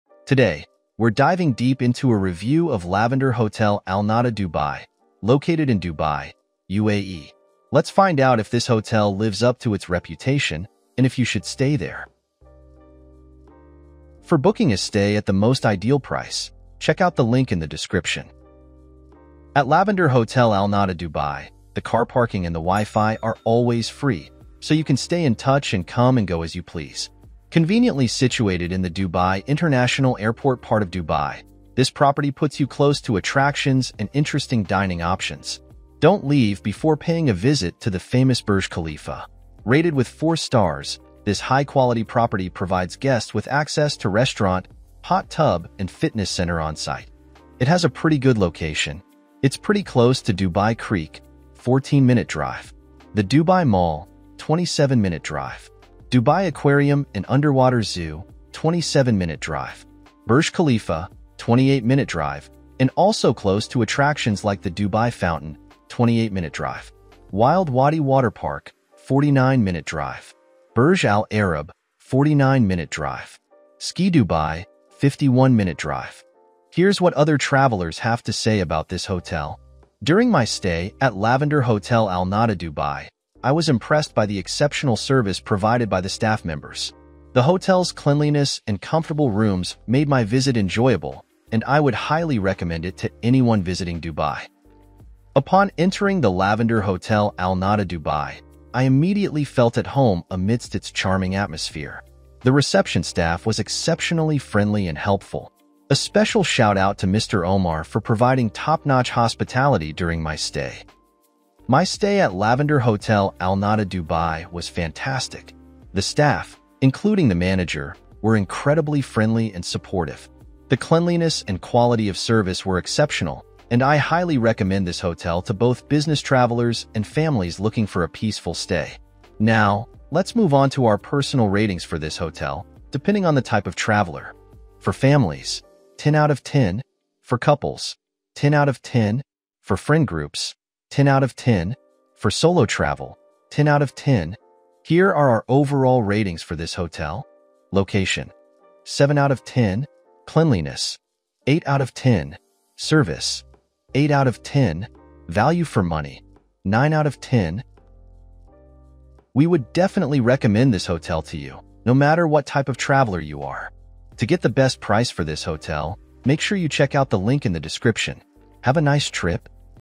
Lavender Hotel Al-Nahda Dubai Review - Is This Hotel Worth It?
In this video, we reviewed the Lavender Hotel Al-Nahda Dubai based on its price, quality, value for money, and more. We also gave it ratings based on the travel type and more.

Additionally, we'll provide you with insights into the nearby attractions and activities that you can enjoy during your stay at Lavender Hotel Al-Nahda Dubai. Whether it's exploring the local culture, sightseeing, or shopping, we've got you covered.